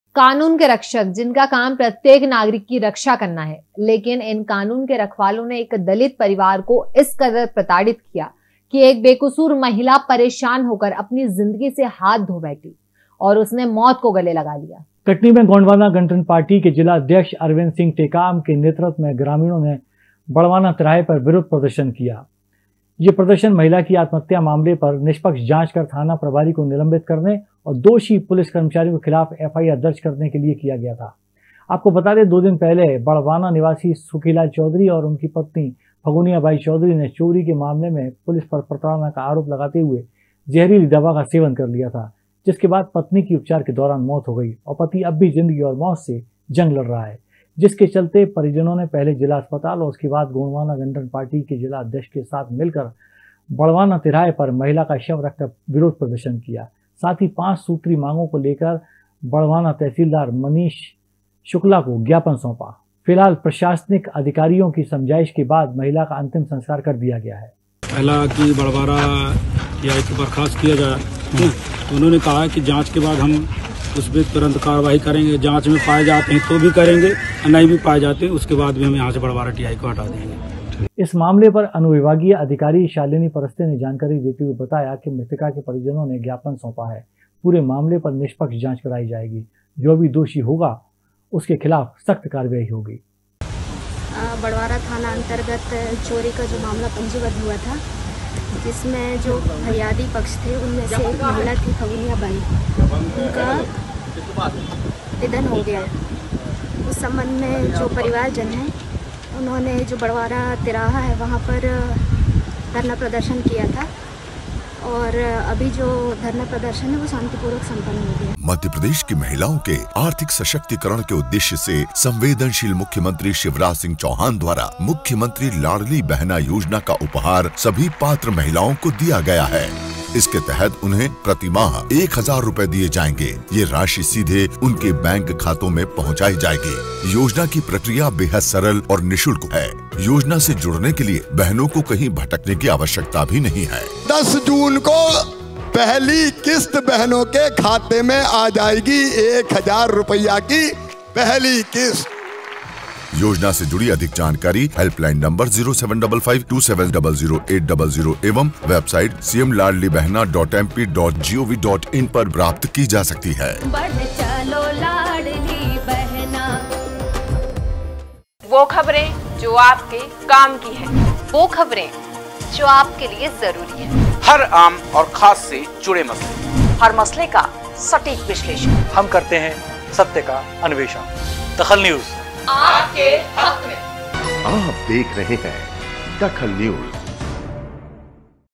DAKHAL NEWS शव रखकर ग्रामीणों ने किया पर धरना प्रदर्शन
शव रखकर ग्रामीणों ने किया पर धरना प्रदर्शन
ग्रामीणों ने प्रशासन से निष्पक्ष जांच की मांग की

news
today news
daily news
protest
latest updates
madhyapradesh news